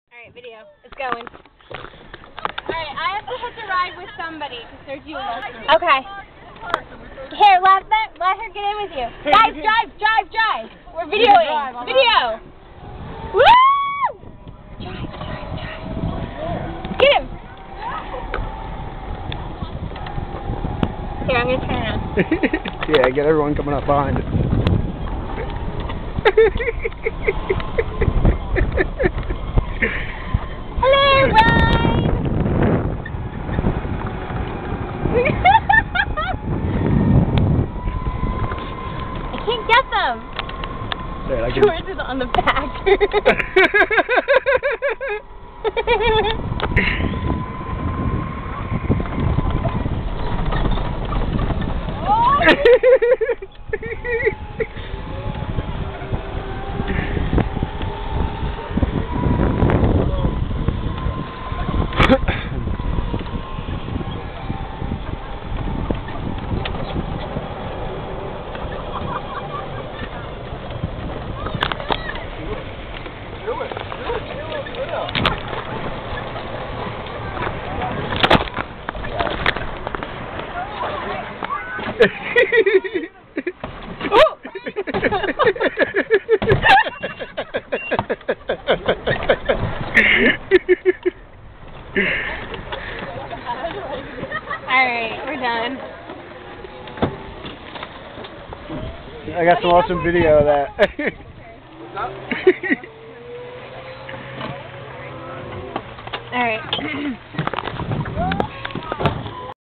wedding party golf cart madness
like Mario Kart but with full length dresses and Tuxedos

ignore the narration- pay attention to Beck's laughter

you may become nauseous from the camera action